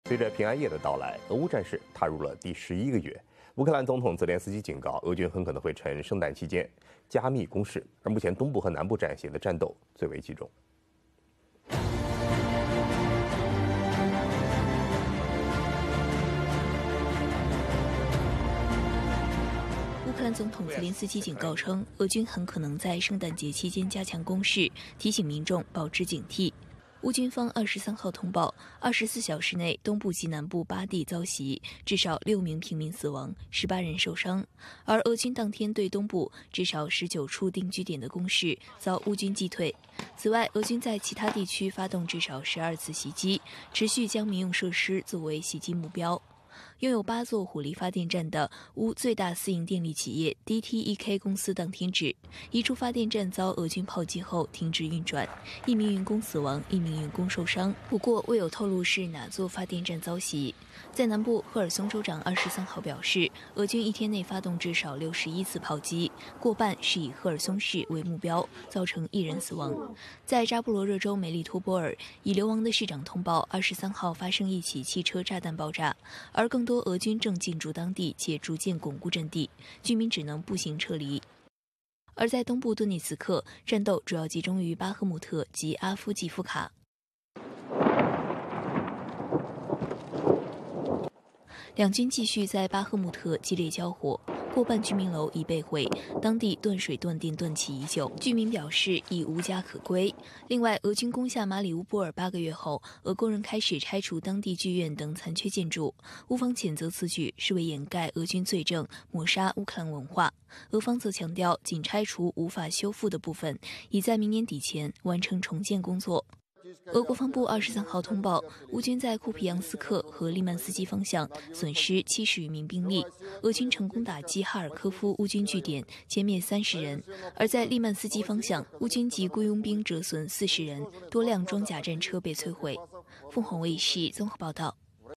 12月24日俄烏戰況｜平安夜俄烏戰火未熄 烏克蘭總統：俄軍或加緊攻勢
烏軍方23日通報，24小時內東部及南部8地遭襲，至少6名平民死亡、18人受傷，而俄軍當天對東部至少19處定居點的攻勢遭烏軍擊退。此外，俄軍在其他地區發動至少12次襲擊，持續將民用設施作為襲擊目標。

赫爾松：
州長23日表示，俄軍一天內發動至少61次炮擊，過半是以赫爾松市為目標，造成1人死亡。

扎波羅熱州梅利托波爾：
已流亡的市長通報，23日發生一起汽車炸彈爆炸，而更多俄軍正進駐當地且逐漸鞏固陣地，居民只能步行撤離。

頓涅茨克：
戰鬥主要集中於巴赫穆特及阿夫季夫卡。兩軍繼續在巴赫穆特激烈交火，過半居民樓被毀，當地斷水斷電斷氣已久，當地居民表示已無家可歸。

另外，俄軍攻下馬里烏波爾8個月後，俄工人開始拆除當地劇院等殘缺建築，烏方譴責此舉是為掩蓋俄軍罪證、抹殺烏克蘭文化，俄方則強調，僅拆除無法修復的部分，以在明年底前完成重建工作。

俄國防部23日通報，烏軍在庫皮揚斯克和利曼斯基方向，損失70餘名兵力。俄軍成功打擊哈爾科夫烏軍據點，殲滅30人。而在利曼斯基方向，烏軍及僱傭兵折損40人，多輛裝甲戰車被摧毀。